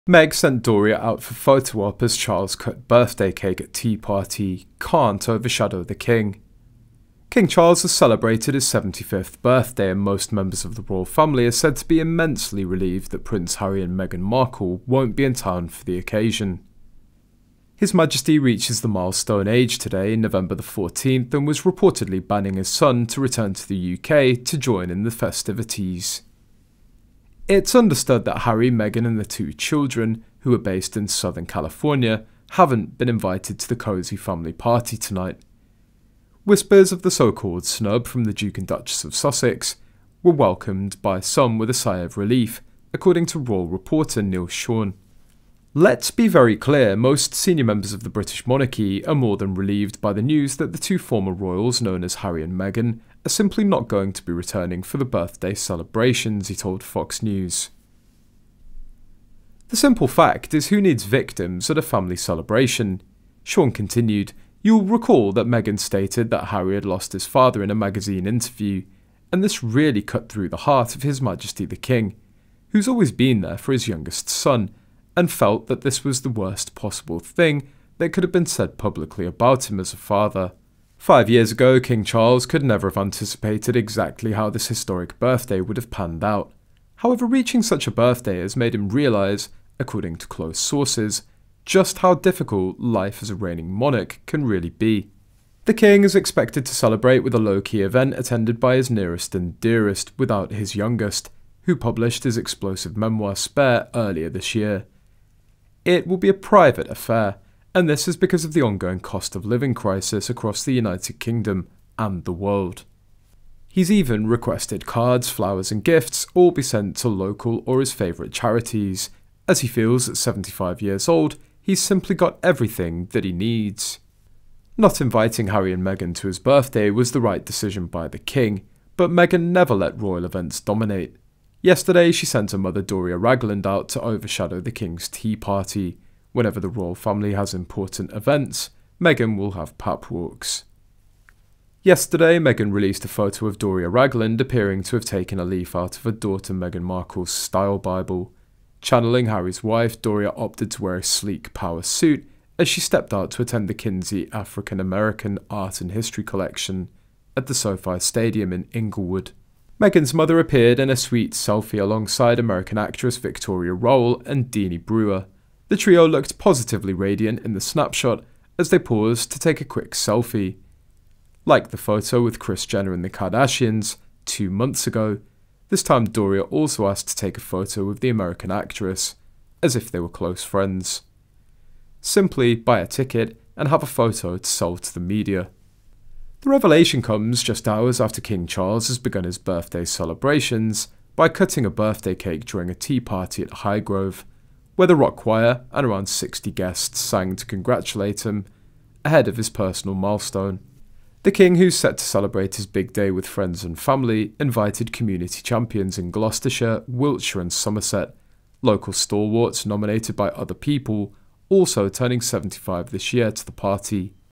Meg Sent Doria Out For Photo-Op As Charles Cut Birthday Cake At Tea Party: Can't Overshadow The King
King Charles has celebrated his 75th birthday – and most members of the Royal Family are said to be immensely “relieved” that Prince Harry and Meghan Markle won’t be in town for the occasion.